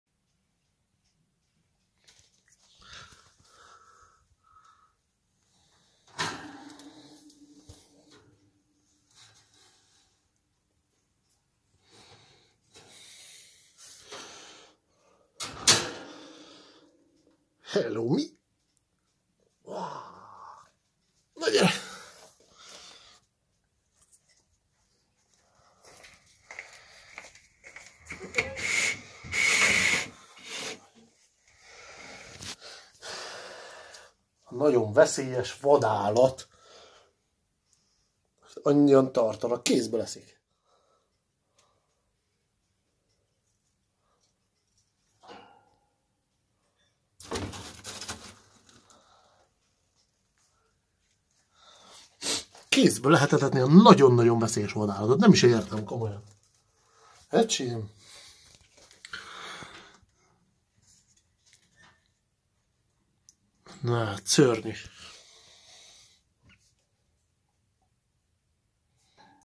Boa hand eating/ Boa kézből eszik
Boa kézből etetés. A nagyon veszélyes állat etetése kézből. Persze a hozzá nem értő embereknek veszélyes. A BUTA embereknek!!!